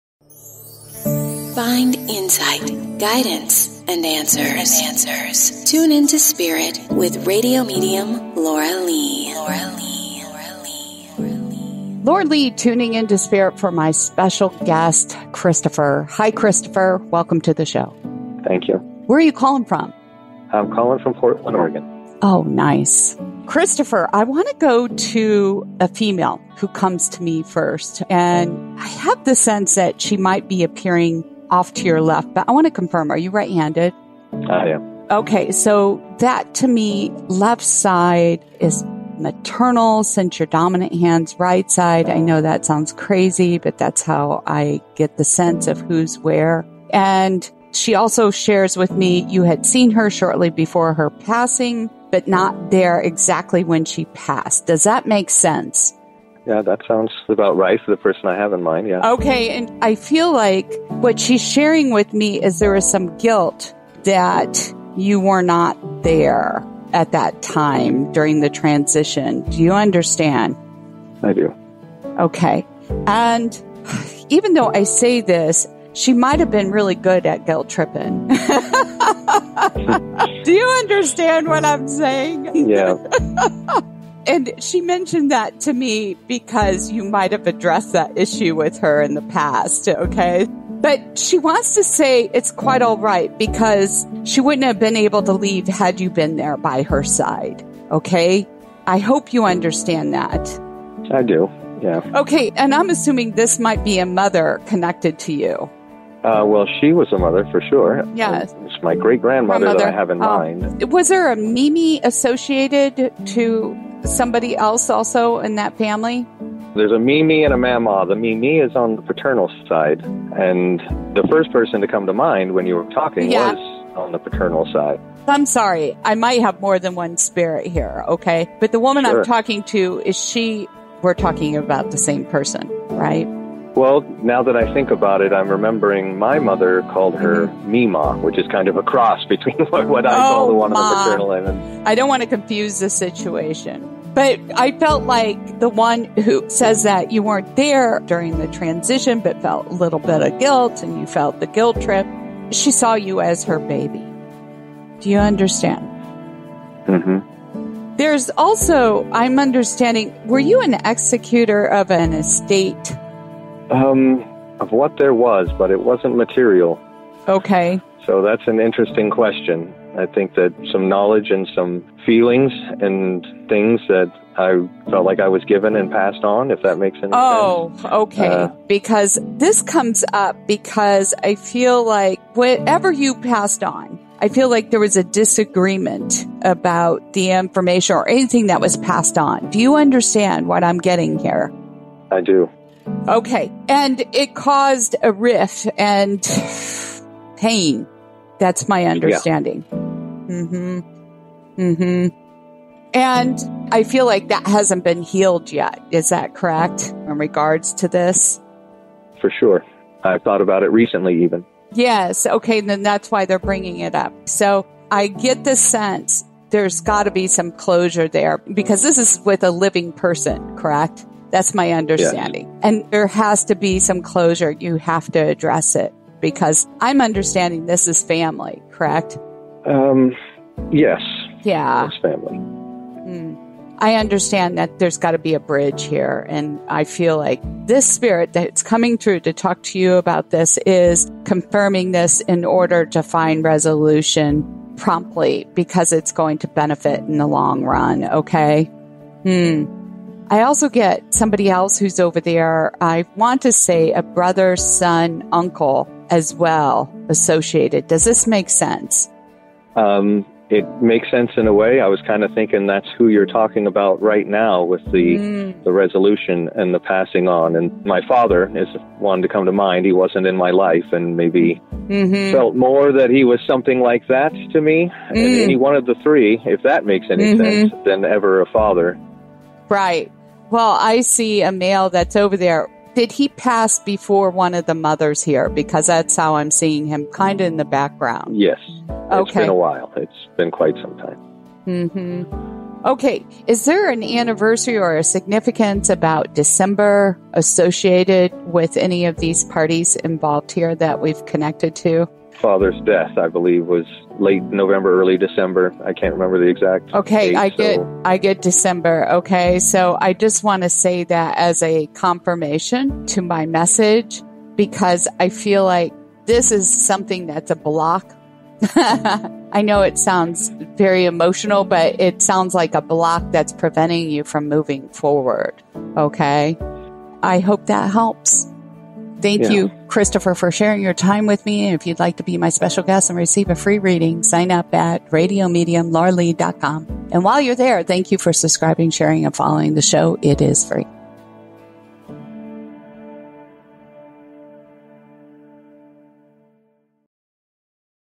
"Guilt trip"
Special guest Christopher hears from his Mimi that it's okay to let go of the guilt.